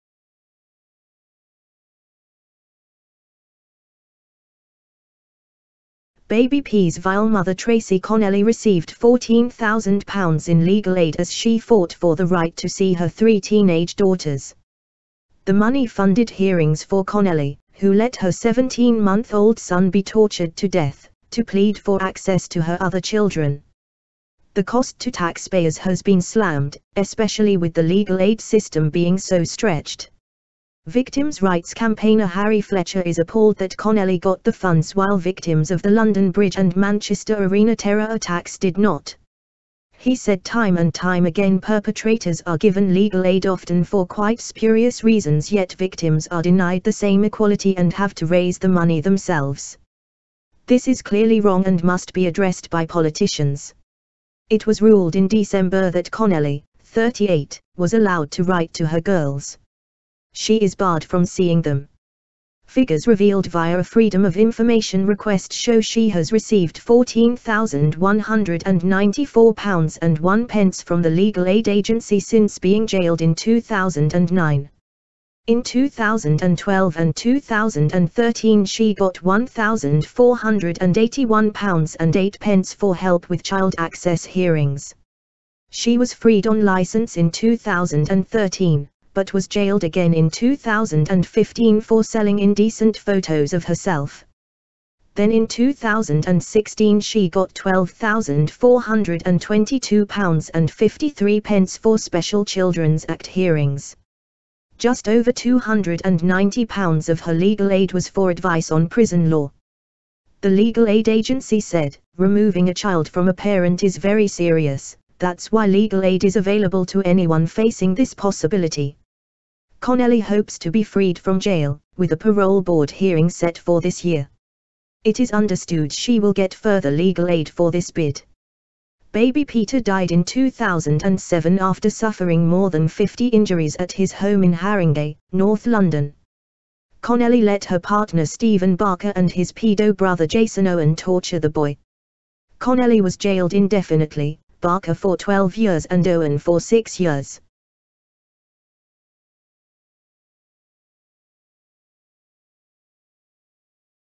Baby P's evil mum given £14,000 in legal aid
Baby P’s evil mother Tracey Connelly received £14,000 in legal aid as she fought for the right to see her three teenage daughters.

The money funded hearings for Connelly, who let let her partner Steven Barker and his paedo brother Jason Owen torture the 17 month old to death, to plead for access to her other children.